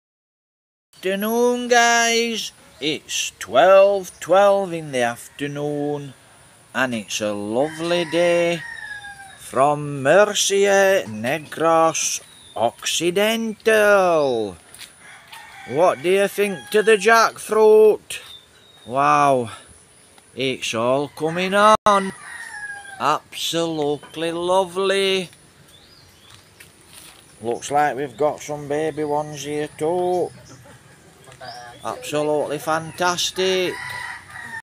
😘Good afternoon Philippines Murcia Province 🇵🇭
Lovely Day Guys Rainy Day Here in Murcia Negros Occidental province Life